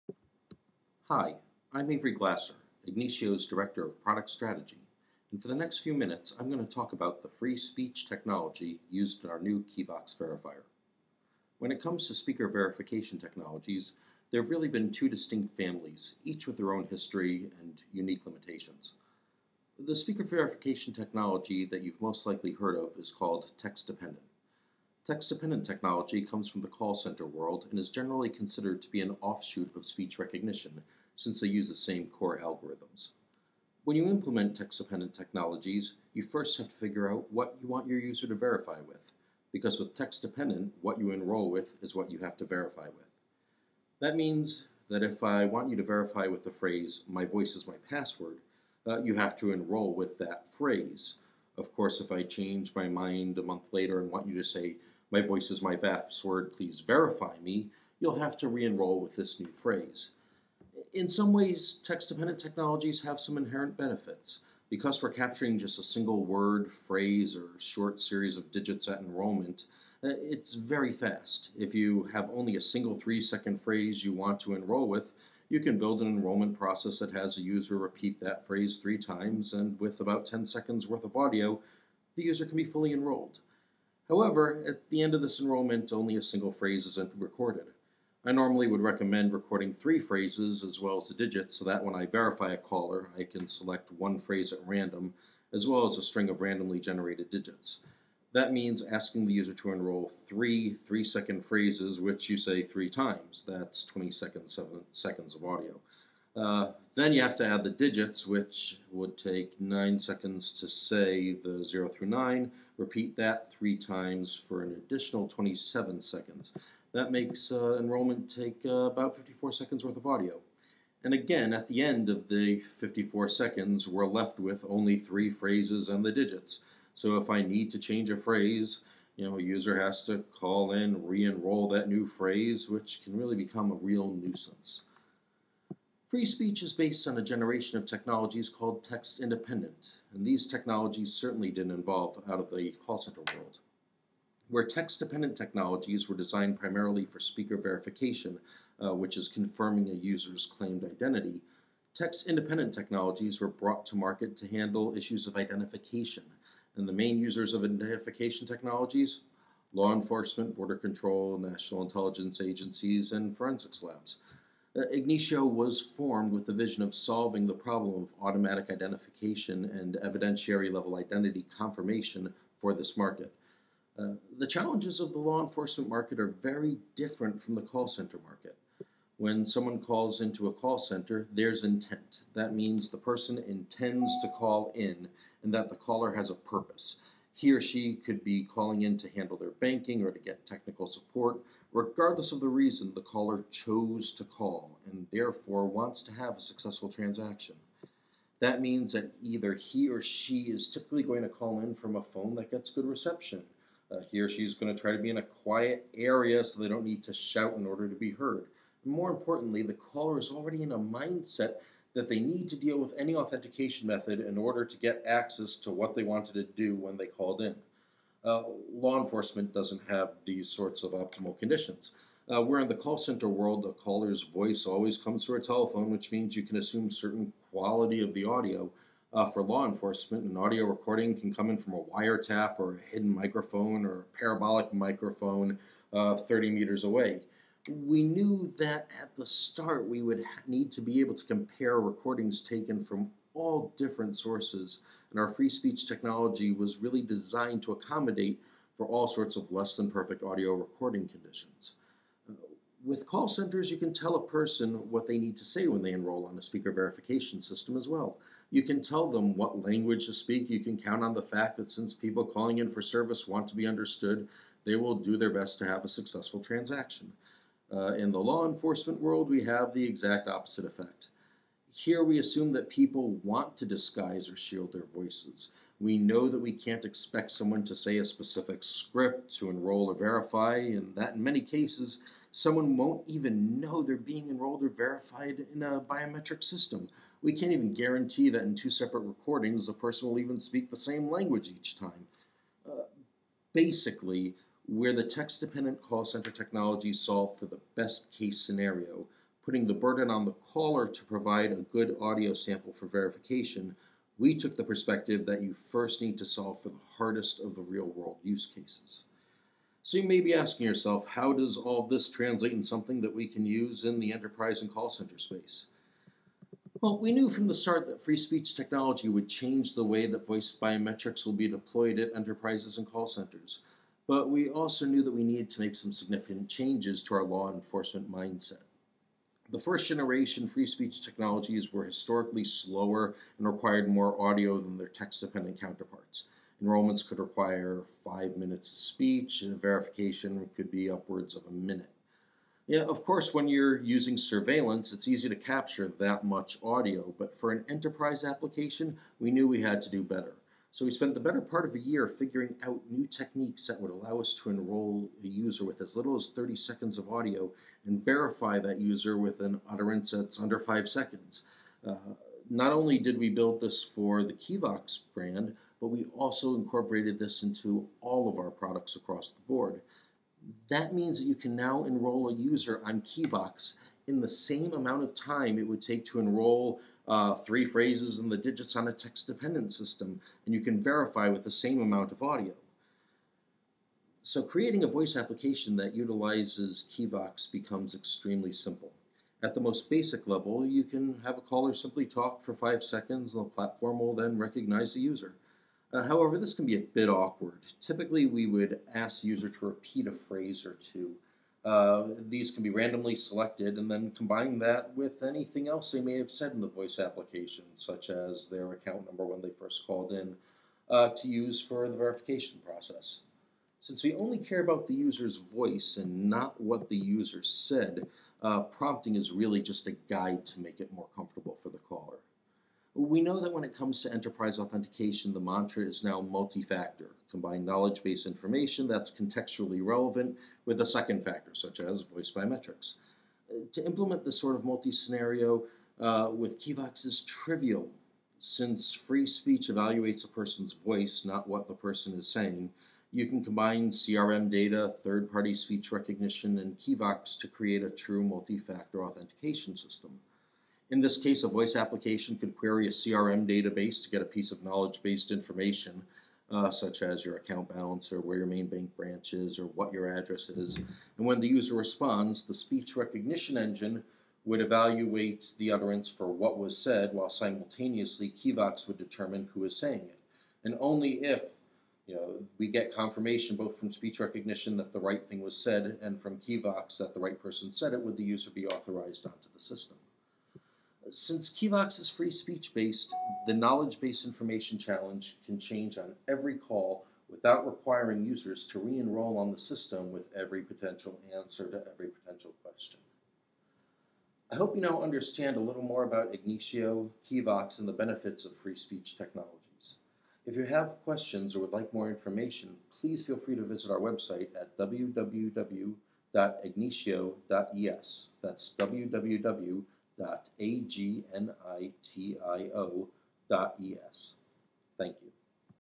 Understanding KIVOX Free-Speech Verifier
A quick primer on Agnitio's KIVOX Free-Speech Verification Solution